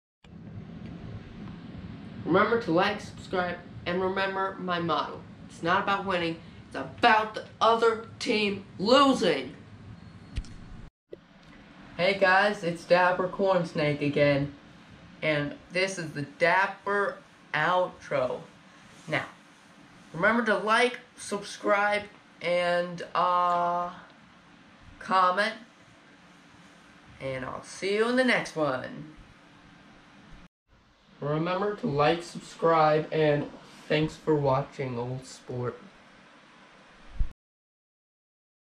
Every Cornsnake Outro Ever
I have assembled all my outros in this video for your viewing pleasure!!!